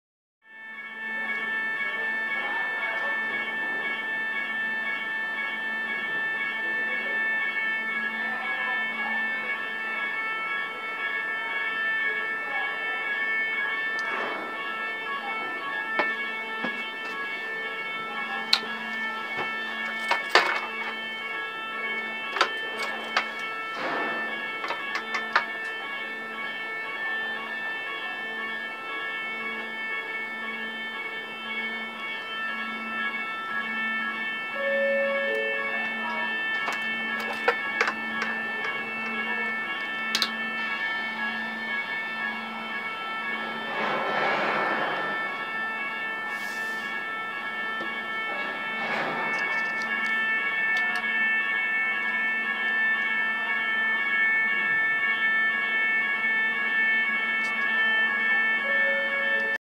Ferry doors opening to let coach off in Full HD Quality
First off the ferry